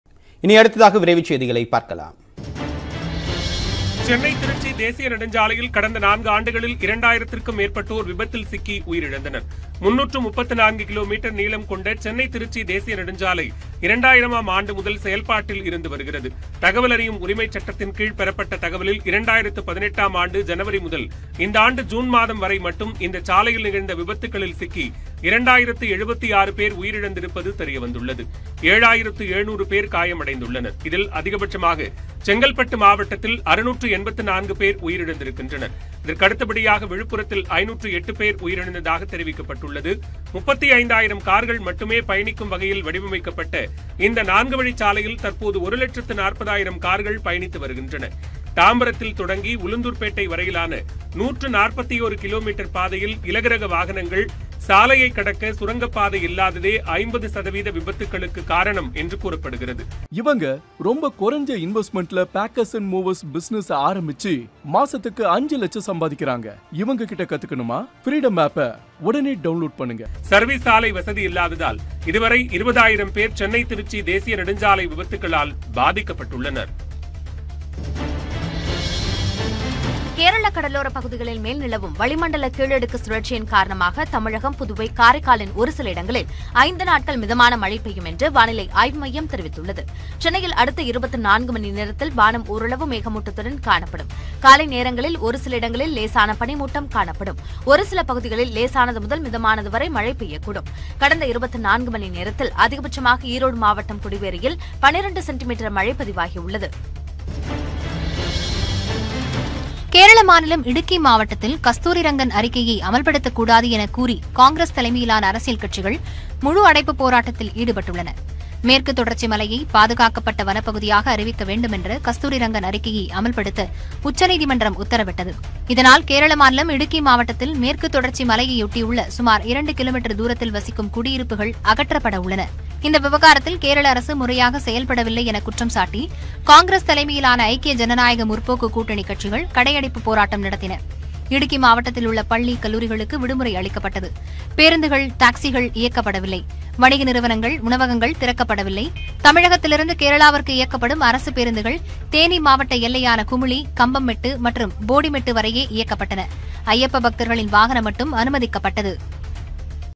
Chennai -Trichy Highway | மரண சாலையாகும் சென்னை - திருச்சி தேசிய நெடுஞ்சாலை | Road Accdient
அதிகளவு மரணங்களை ஏற்படுத்தும் சென்னை-திருச்சி தேசிய நெடுஞ்சாலை. இதற்கான காரணத்தை விவரிக்கிறது இந்த செய்தி தொகுப்பு